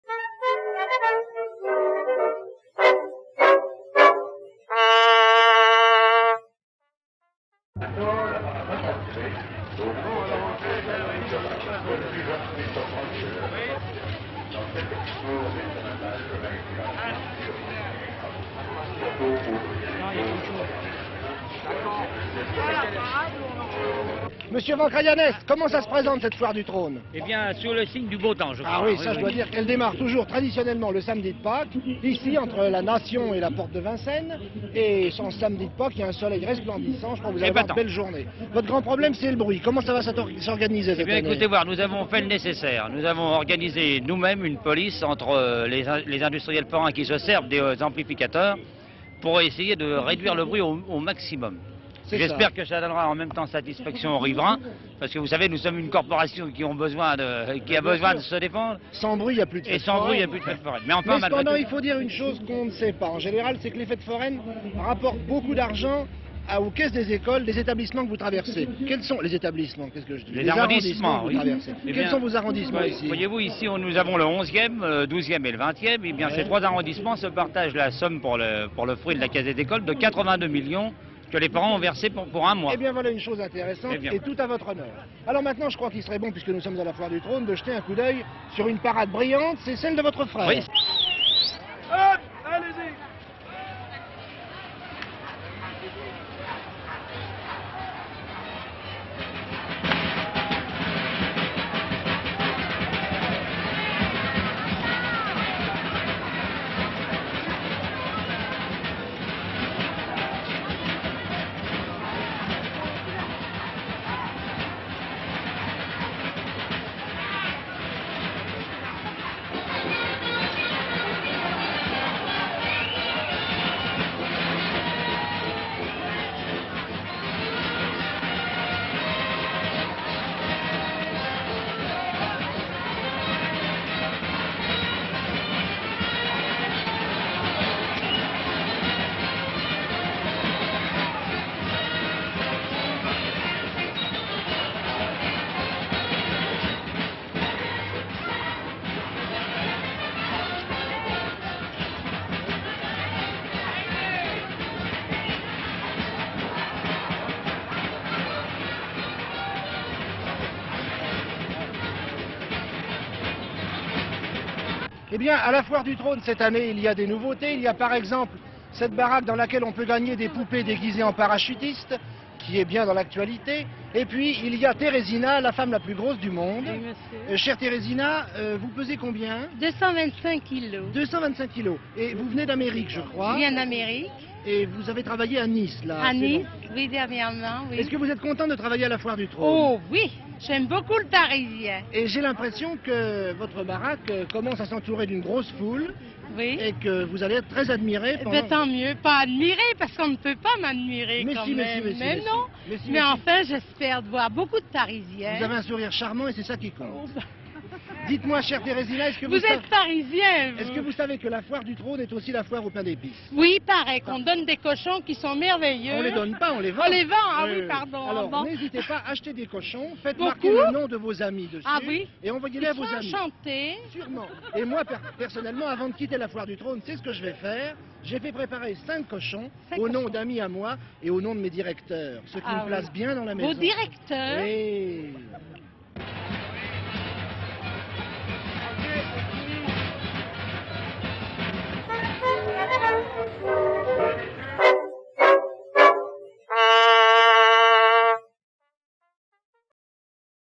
Pierre Tchernia à la Foire du Trône | Archive INA
28/03/1959
Reportage de Pierre TCHERNIA à la Foire du trône à Vincennes.
  Pierre TCHERNIA interviewe un organisateur au milieu de la foule puis TERESINA (la femme la plus grosse du monde : 240 kgs).